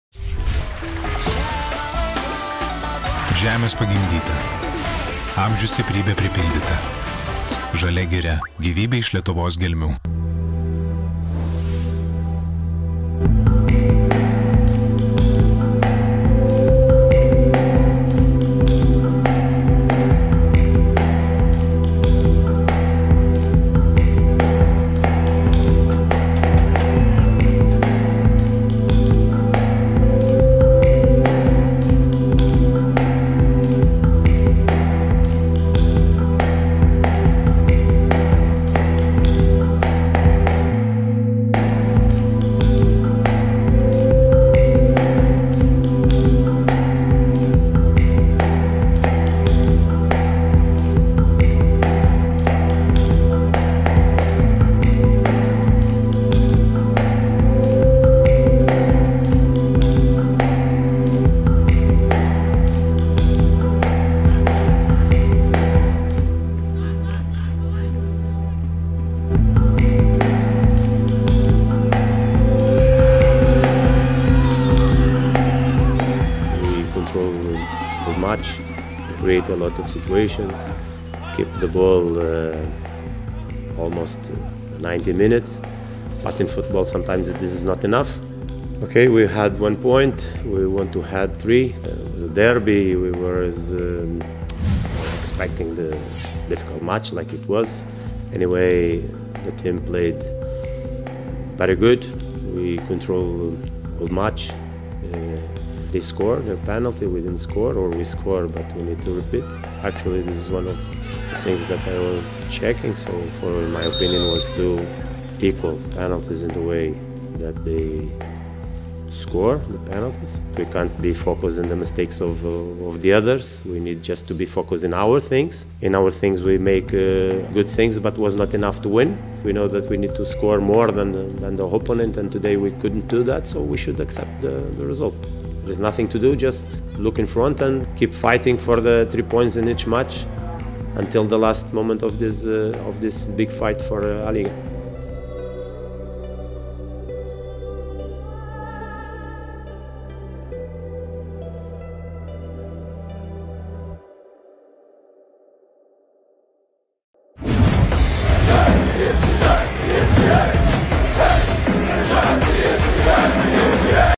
J.Martinsas: „Baudiniai buvo identiški“
Joao Martinsas po A lygos 24-ojo turo rungtynių su „Riteriais“ turėjo ką pasakyti.